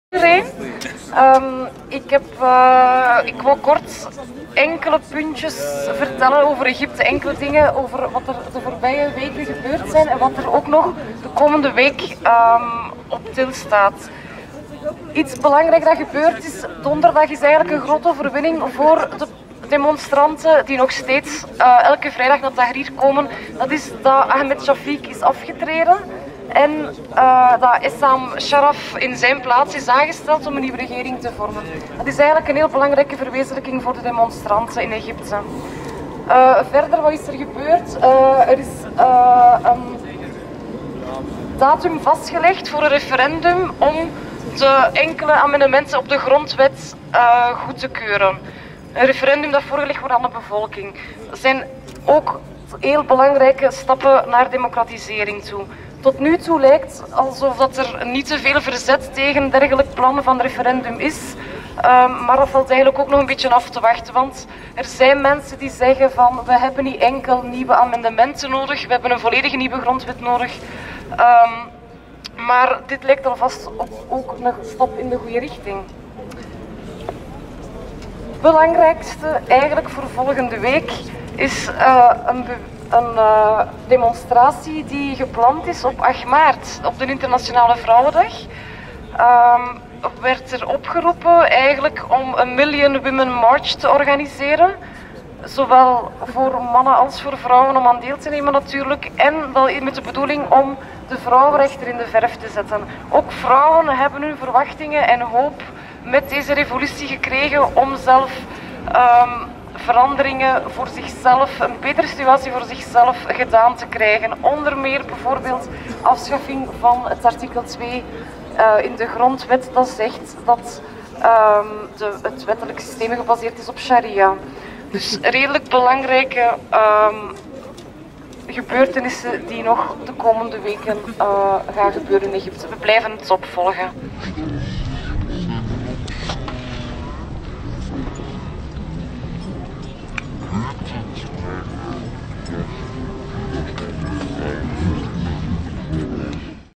Solidariteitsgroep te Gent 2011
Dank aan Thomas W . SAP.

Wij eisen:
De vrijlating van alle politieke gevangen in de regio.
Geen immenging door de buitenlandse economische belangen en instelling.
De bevriezing van de vermogens en bezettingen van de Arabische dictator door de belgische regering en de teruggave ervan aan de burgers van de regio.
De kwijtschelding van de schuldenlast.
Wij zijn solidair met de zelforganisatie van de volkeren, de volkcomités, de vakbonden en de democratische bewegingen in hun strijd voor sociale rechtvaardigheid en democratische rechten.

Wij roepen ook op voor de Nationale Solidariteitsbetoging op zondag 20 maart 2011, in Brussel. vertrek: Noordstation.